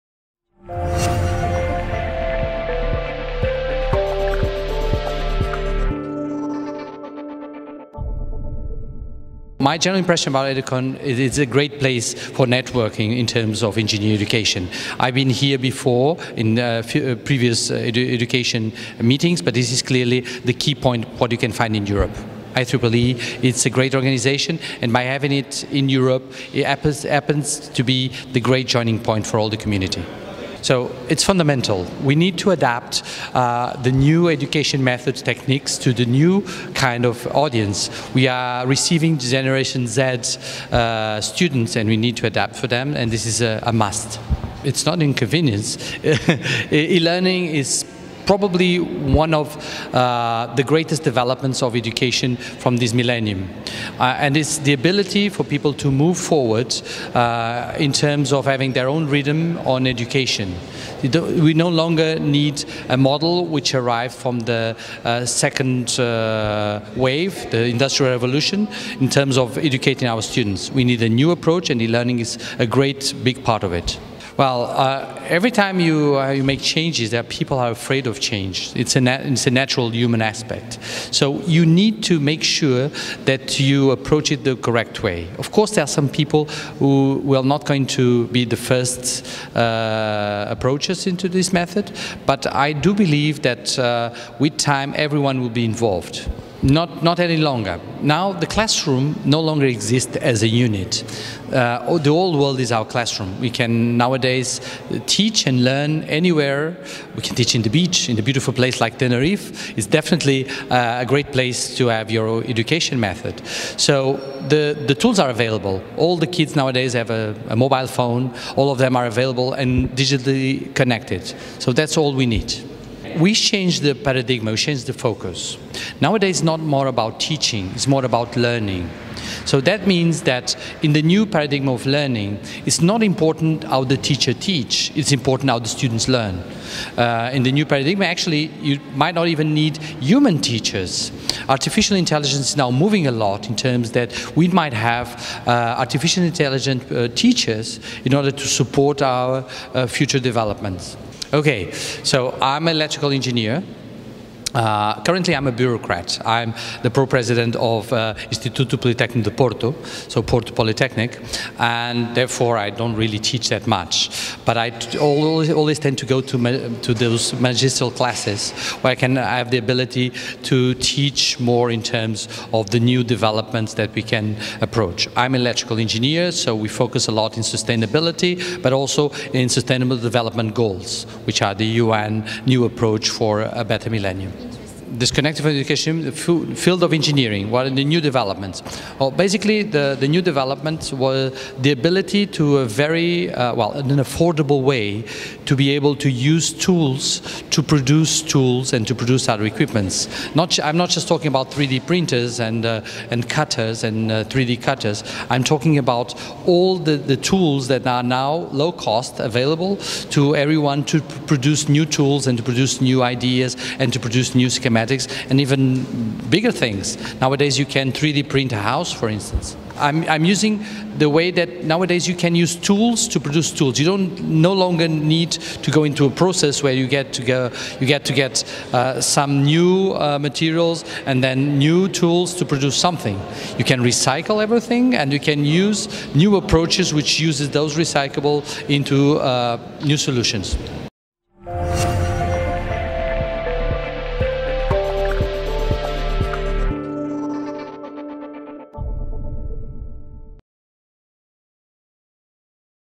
Interview: Quadrado at EDUCON 2018
The IEEE Global Engineering Education Conference (EDUCON) 2018 is the ninth in a series of conferences that rotate among central locations in IEEE Region 8 (Europe, Middle East and North Africa). EDUCON is the flagship conference of the IEEE Education Society.